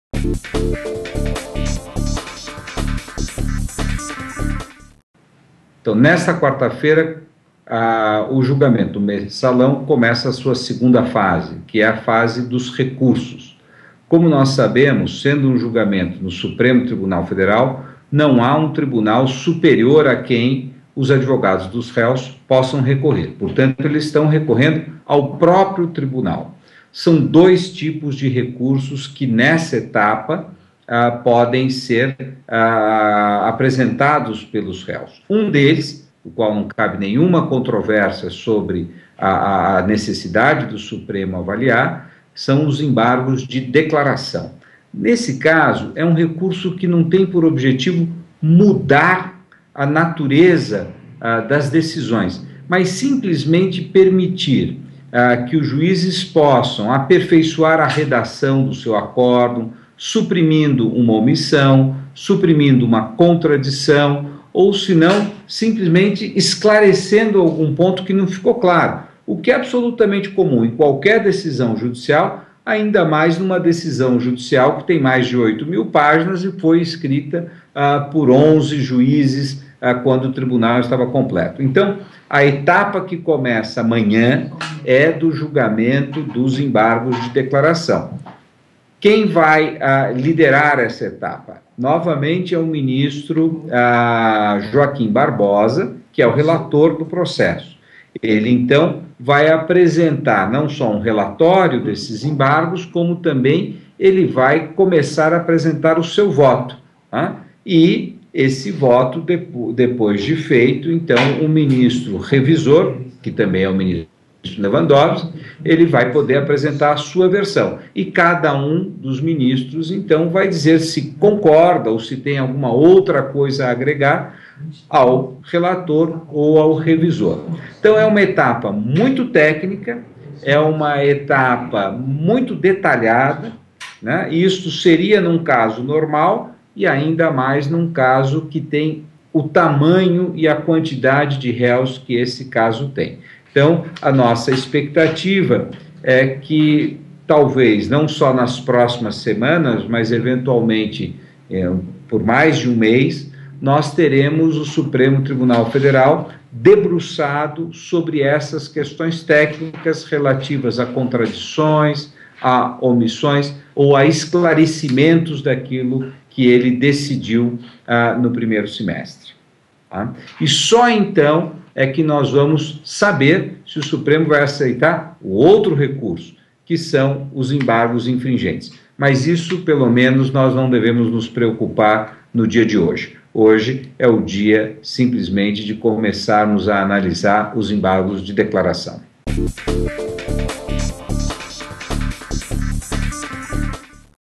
Entenda a retomada do julgamento do mensalão
Oscar Vilhena, professor da Direito GV, comenta os embargos de declaração, primeiro tipo de recurso a ser julgado pelos ministros nessa nova fase